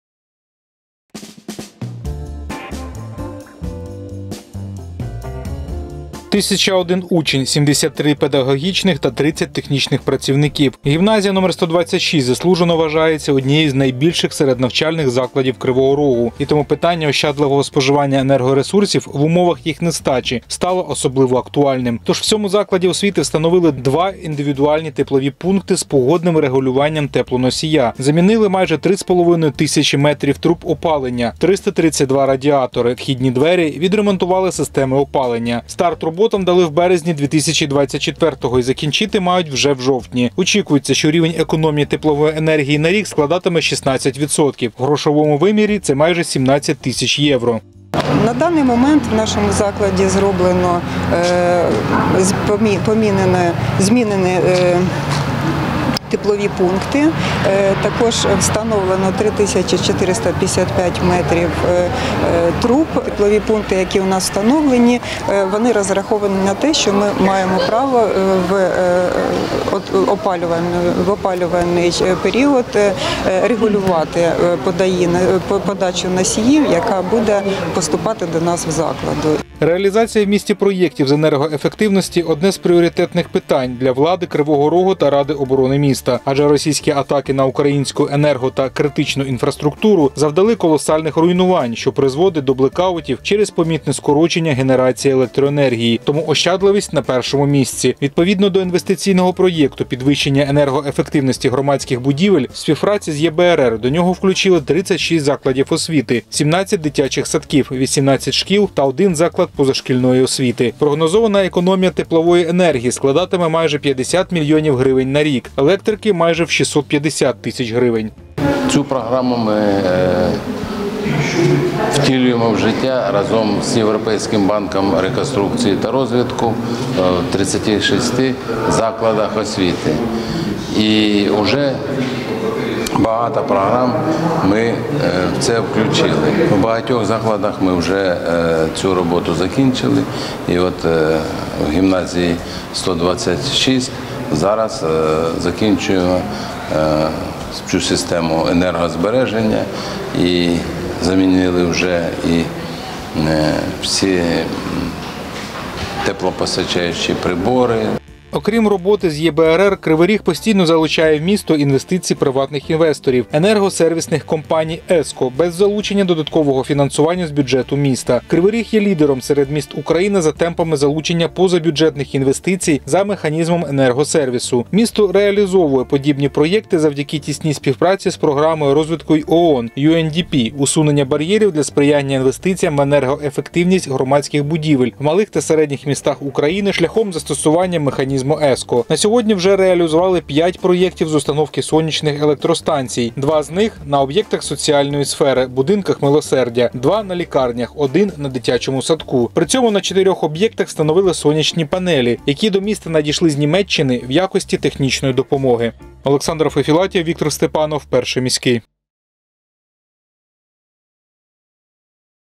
Енергоефективна модернізація в гімназії №126
У Кривому Розі підготовка до опалювального сезону триває Робота над впровадженням проєктів з енергоефективності та посилення енергетичної стійкості комунальних закладів міста в дії. В криворізькій гімназії №126, що на Зарічному, в рамках співпраці з Європейським банком реконструкції та розвитку, встановили нові індивідуальні теплові пункти з погодним регулюванням теплоносія, замінили значну протяжність труб опалення, велику кількість радіаторів, тощо. Пріоритет міської влади в умовах постійних ударів ворога по енергоінфраструктурі та за графіків відключень електроенергії – ощадливе використання енергоресурсів, підсилення автономності, зменшення залежності комунальних закладів від споживання традиційних джерел споживання. Більше розкажуть наші кореспонденти.